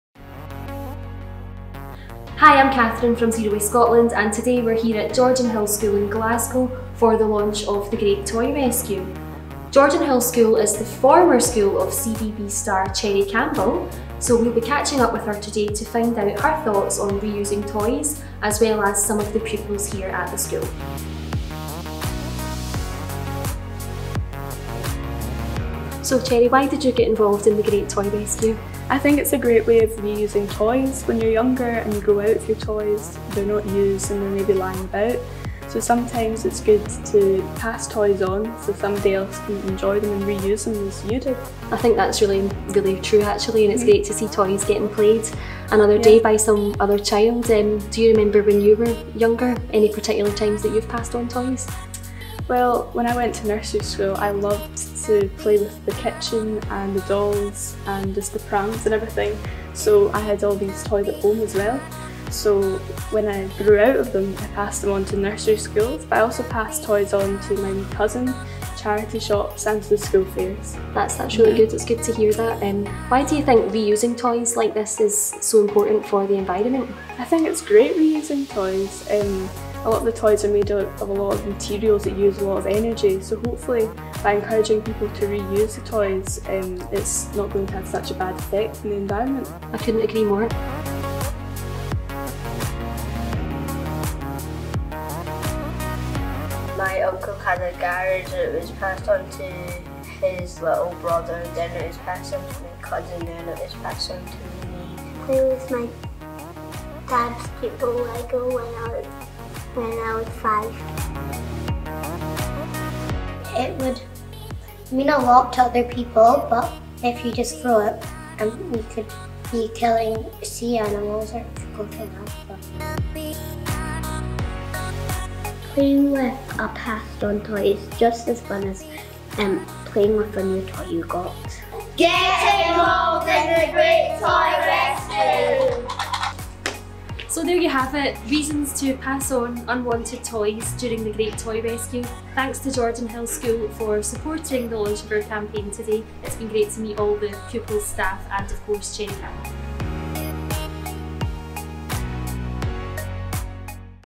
Cherry Campbell, star of hit children's TV show Katie Morag, helps launch The Great Toy Rescue
Cherry Campbell, star of hit children's TV show Katie Morag, helps launch The Great Toy Rescue at her former school Jordanhill in Glasgow. Cherry and current pupils share their thought on reusing toys and turning pre-loved into re-loved.